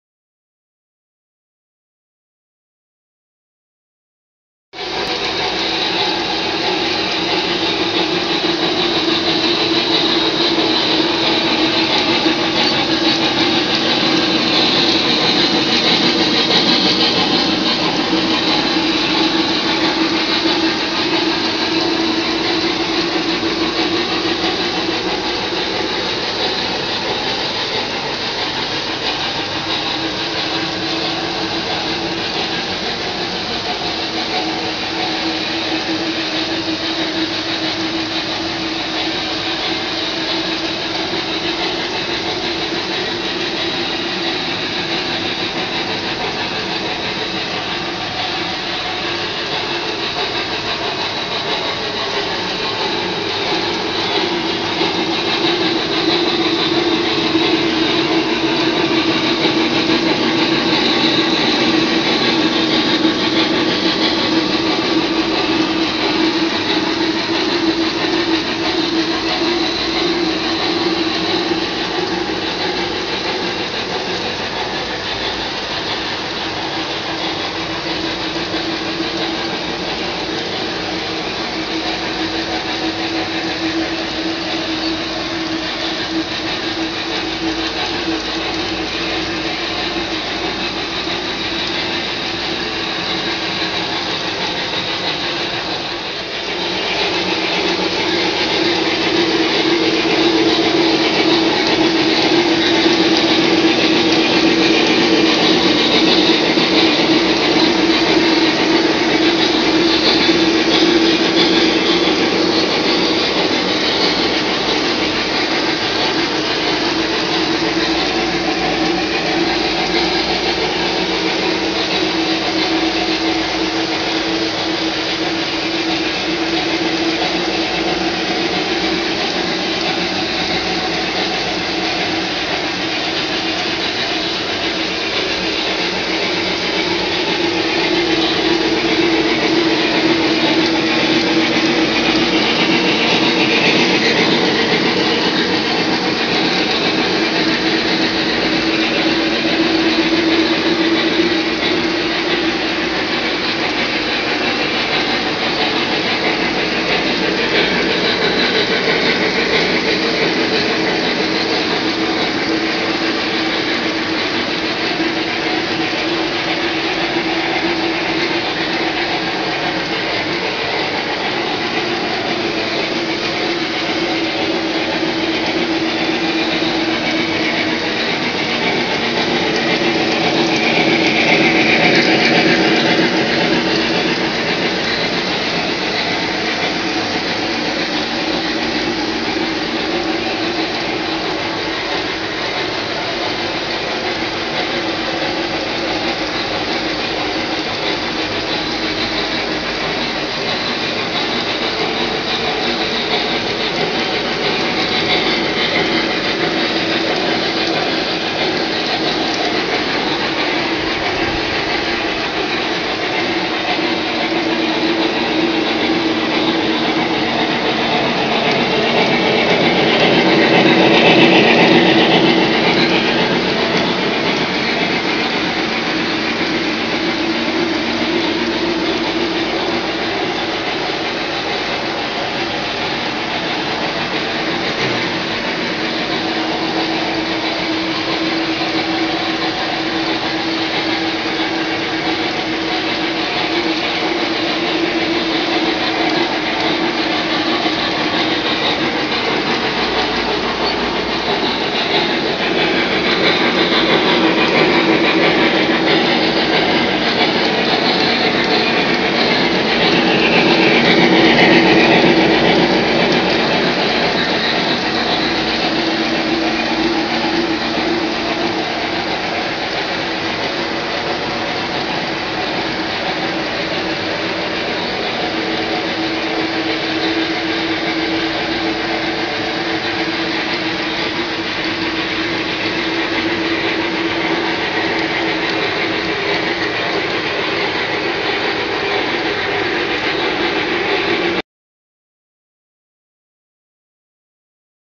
A Sunday Afternoon Running Trains
The engines are the recent Lionel B&O EM-1 and the recent Lionel W&LE USRA Mallet.  The rolling stock is a combination of MTH Premier, Lionel, K-Line, 3rd Rail, Atlas O, and Buffalo Creek Graphics (limited edition custom decorated Weaver cars).